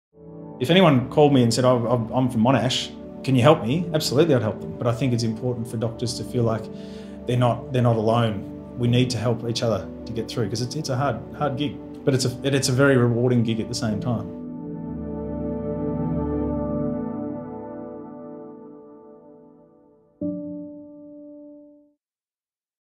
Monash alumni | Dr Adam Castricum
Dr Adam Castricum is a proud Monash alumnus. Adam is currently team doctor for the Hawthorn Football Club and president of the Australasian College of Sport and Exercise Physicians. He's passionate about sharing his knowledge and supporting other Monash doctors.